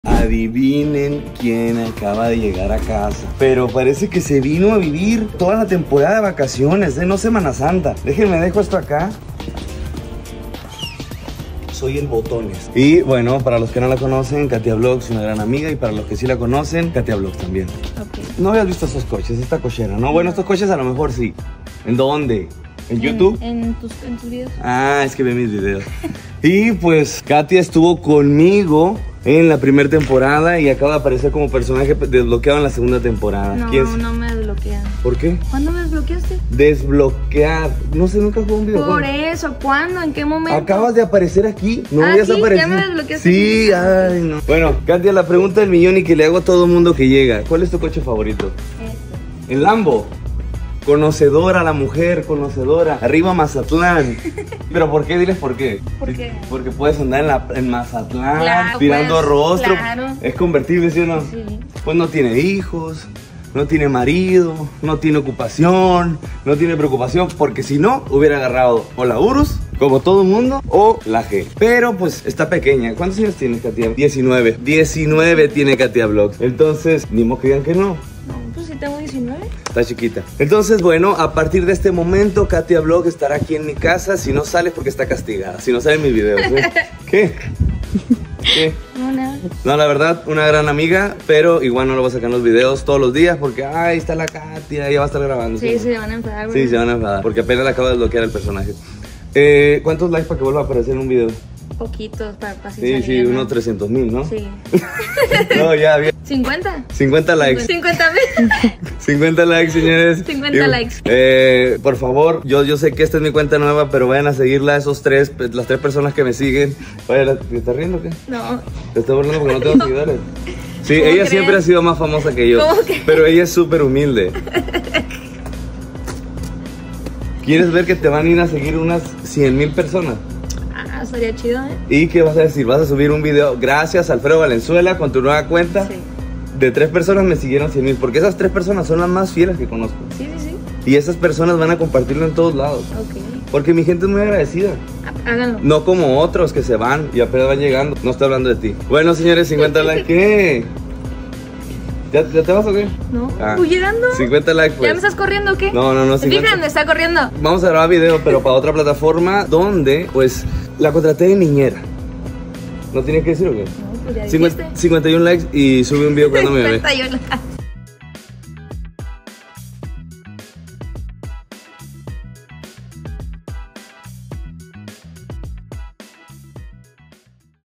Llega de Sorpresa Katia Vlogs!
Katia Vlogs Llega de Sorpresa a La Casa de Alfredo Valenzuela